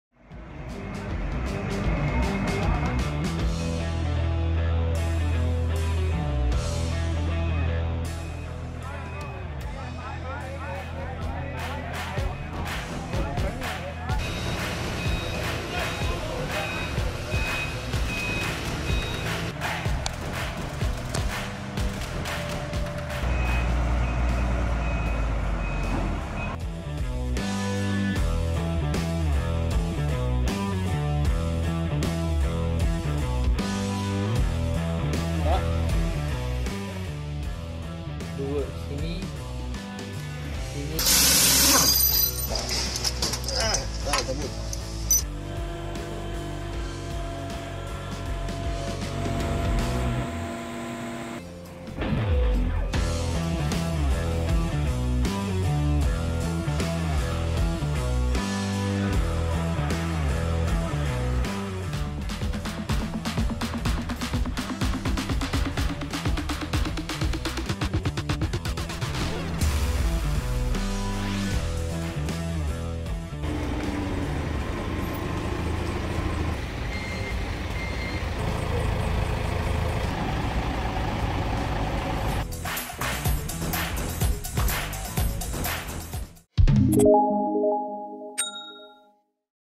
Aliran Industrial Machine's Shifting & Moving Services | Forklift Training | Safety Training 2022
Suria Aliran
Video showing industrial machine moving or shifting services by Aliran Tenaga Technology Sdn Bhd.